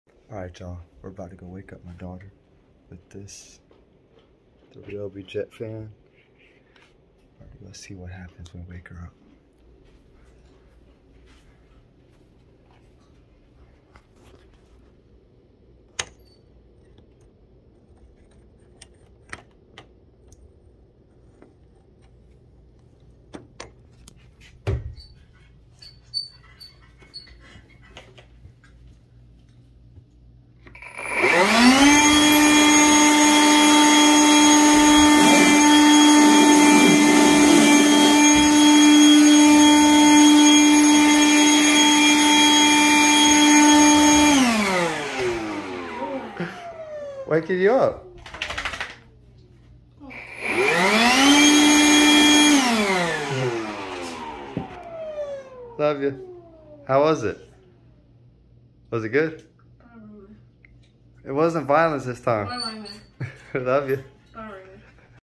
Waking up my daughter with Ryobi HP Brushless Jet fan pt1
My 16 year old daughter likes to sleep all day, so I decided to wake her up with my New Ryobi HP BRUSHLESS JET FAN BLOWER 😂😂